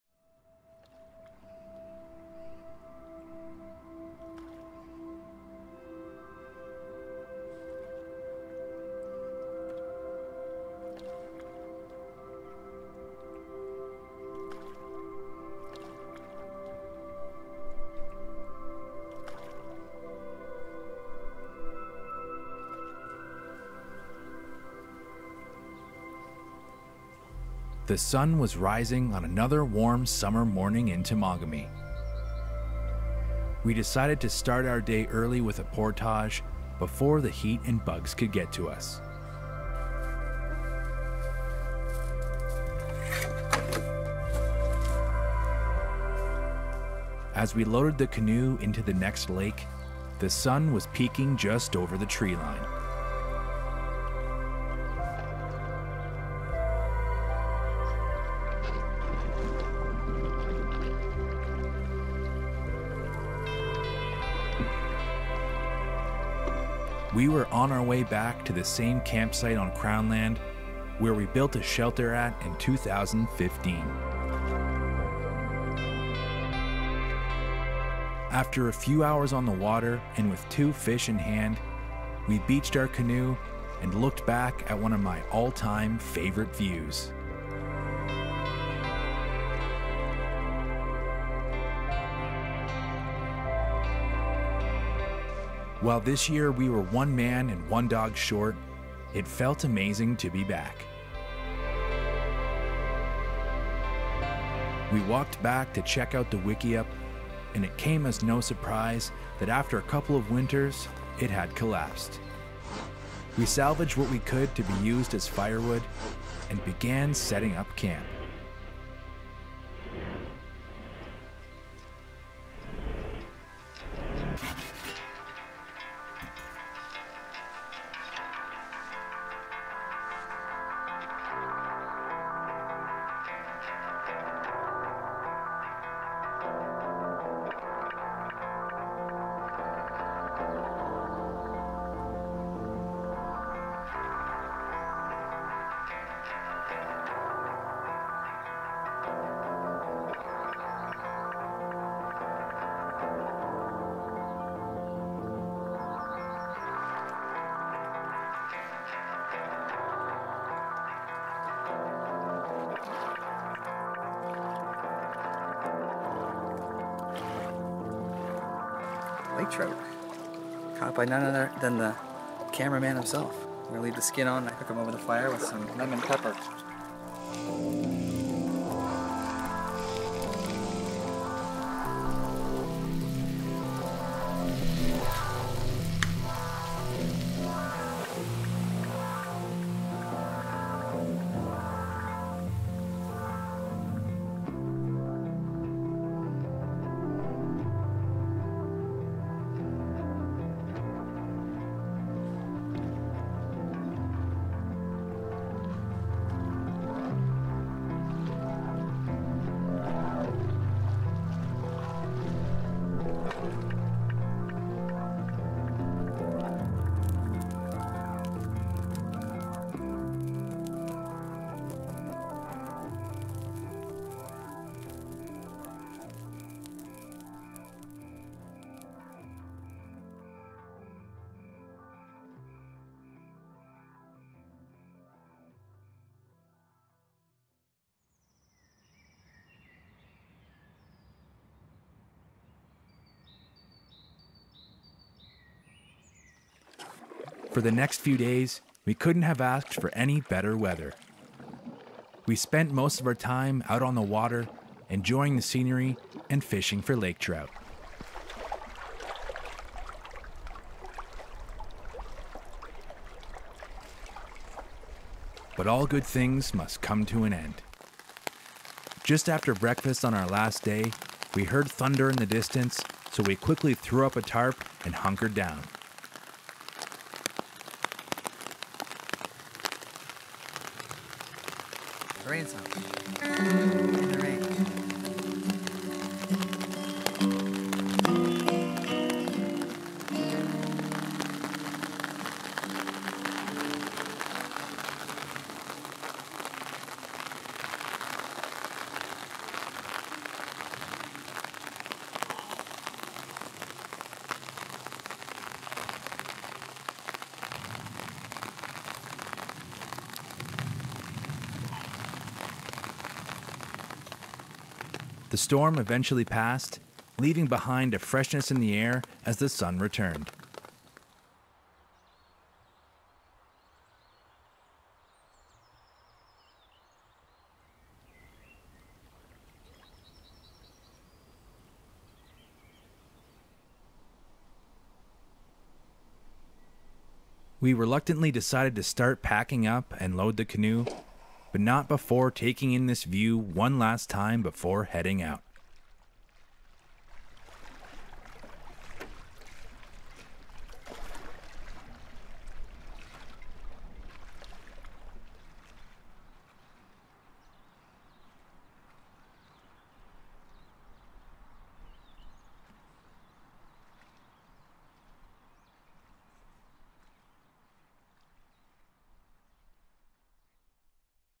A Canoe Trip in Temagami
A taste of summer backcountry canoe vibes in majestic Temagami, Ontario. In the summer of 2018, my best friend Jordan and I paddled back to a campsite on crown land where we built a wikiup shelter at in 2015 with Clint Zold, which can be seen in the short film 'Nature Remains'

This was everything one could hope for on a canoe trip:  beautiful weather, good fishing and some quality friendship.